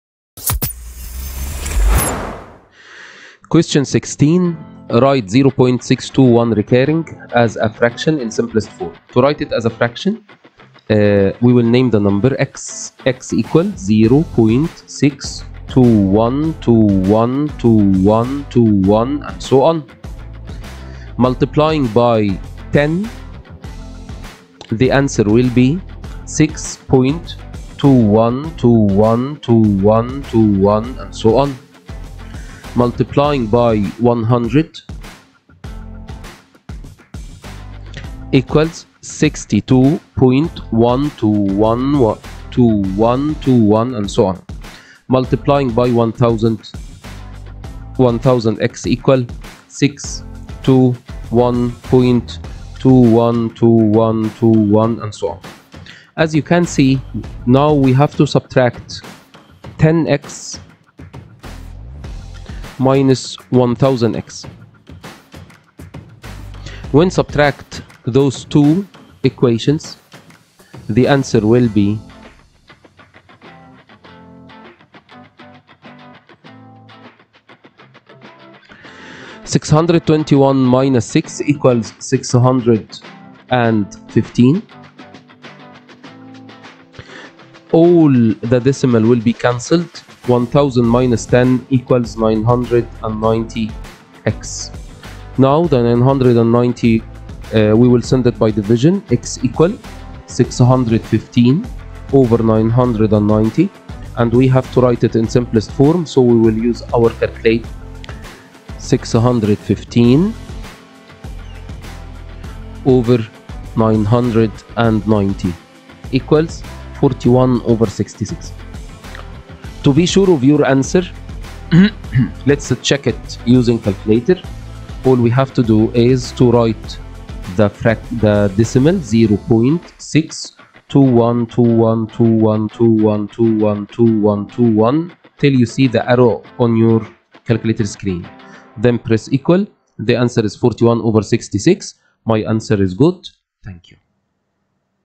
0580 22 m j 23 Q16
All of the IGCSE Mathematics
step-by-step solution for the latest IGCSE exam
In this playlist you should be able to see the following:
Cambridge IGCSE Mathematics 0580/22/M/J/23 discussion and solution
Mathematics 0580 | Paper 2 Variant 2 | May / June 2023 || Full Length
0580/22/m/j/23